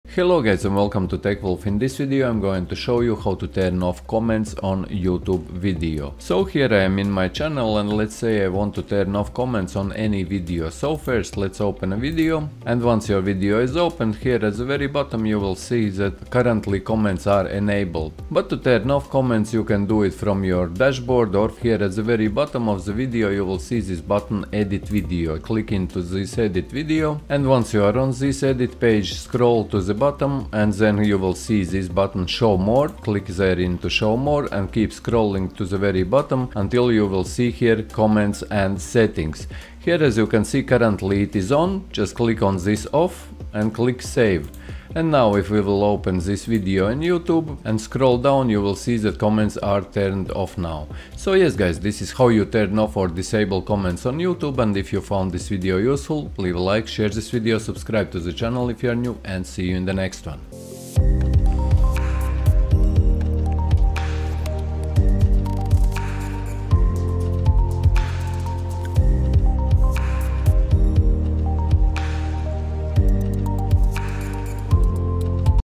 How To Turn Off Comments On YouTube - Disable / Hide YouTube Comments
In this video learn How To Turn Off Comments On YouTube - Disable / Hide YouTube Comments